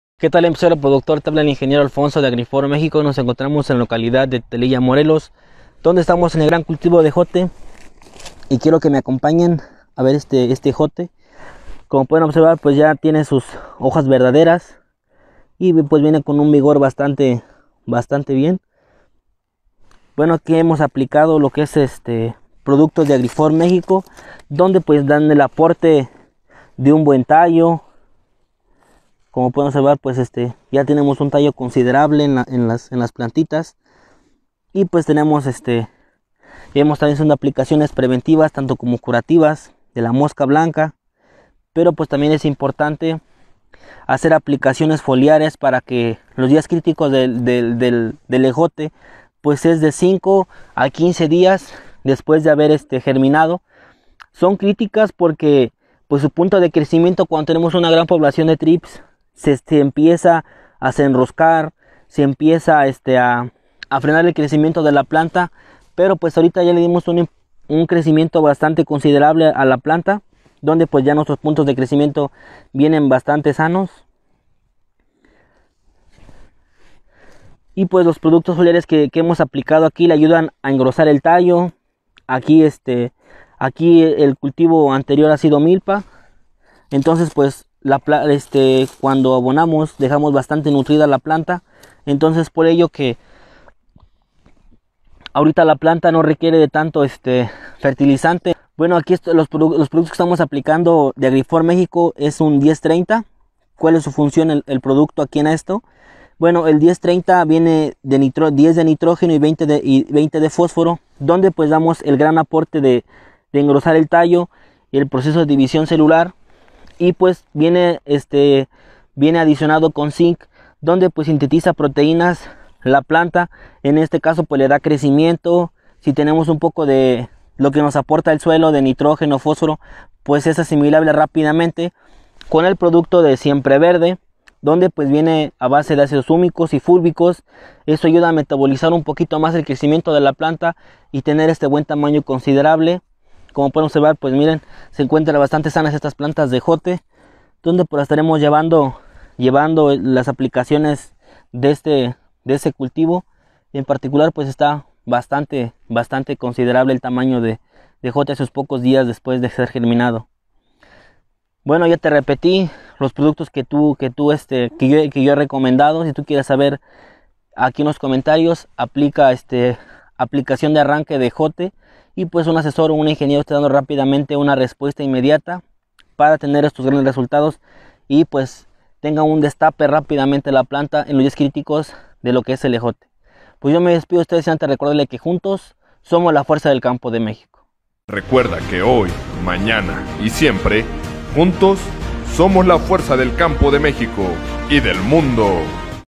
Resultados de crecimiento en ejote💪
EmpresariosProductores nuestras visitas de la RutaAgrifor no paran y en esta ocasión el Ing. Alfonso nos muestra cómo se lograron tener tallos más gruesos en este cultivo de ejote
👇¿Qué productos se aplicaron? Te compartimos la aplicación en los comentarios👇